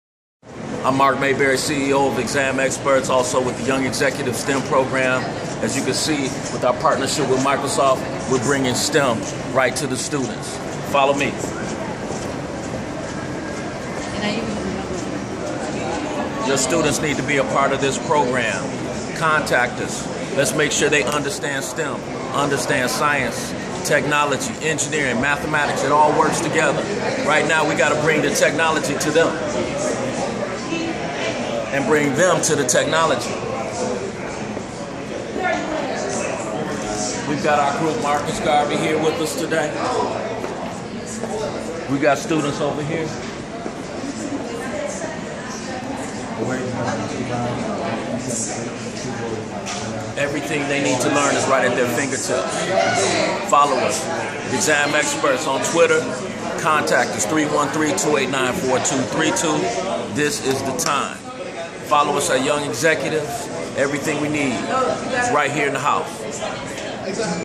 ExamExperts STEM Partnership with Microsoft
Mark J. Mayberry speaks on the exciting new STEM partnership with Microsoft where students get hands-on training on new Microsoft applications at the Microsoft Center in Troy, MI. Follow us on @mayberryspeaks and @examexperts.